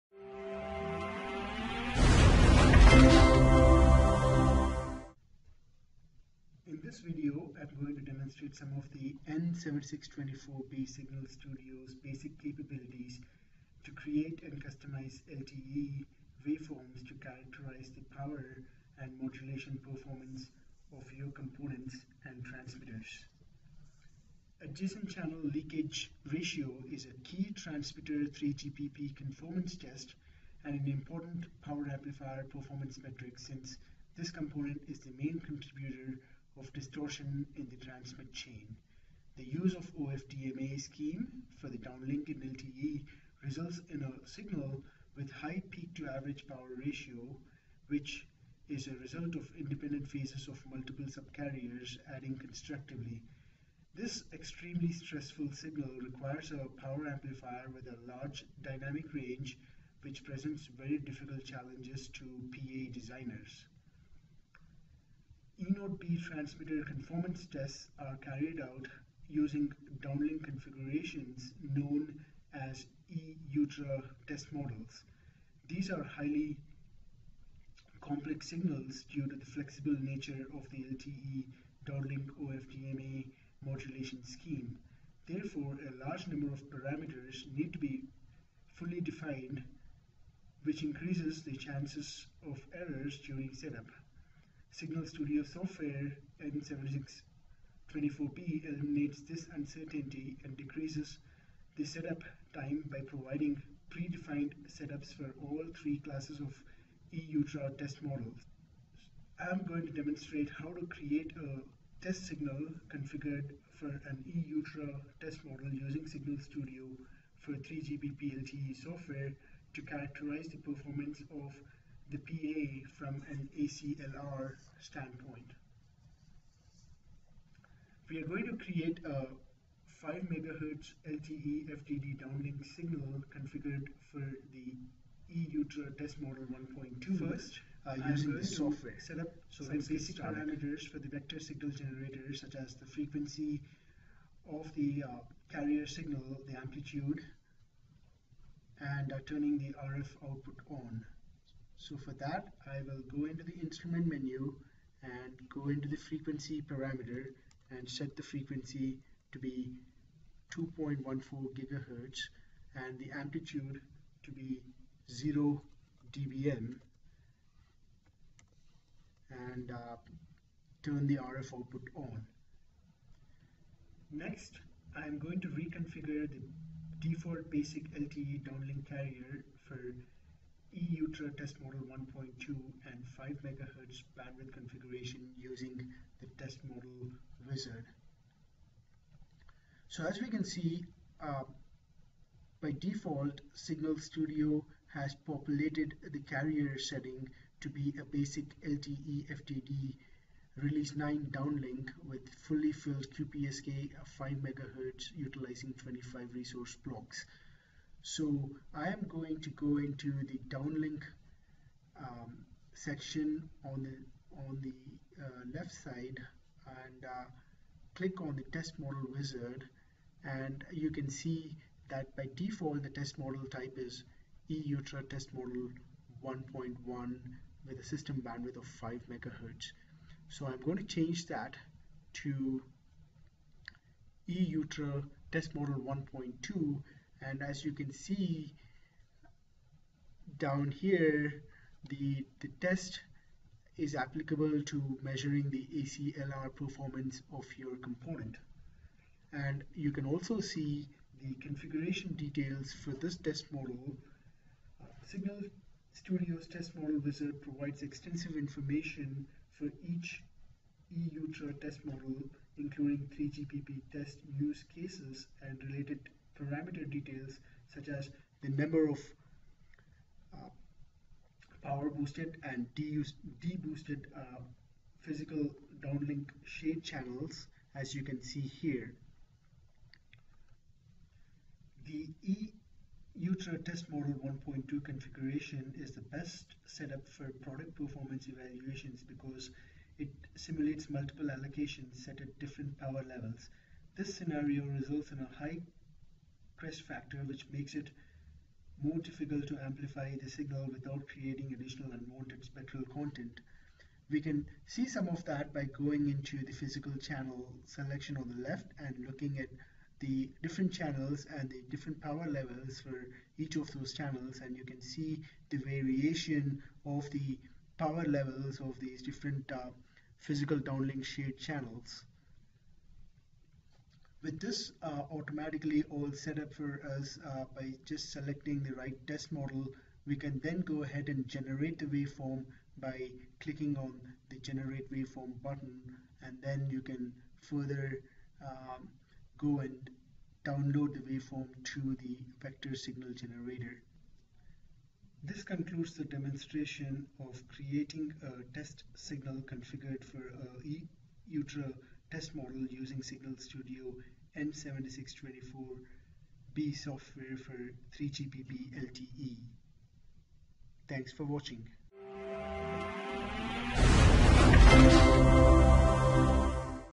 Keysight LTE FDD Signal Studio software N7624B for creating E-TM signals for ACLR characterization
Creation of LTE FDD E-TM Signals for ACLR testing using Keysight Technologies N7624B Signal Studio.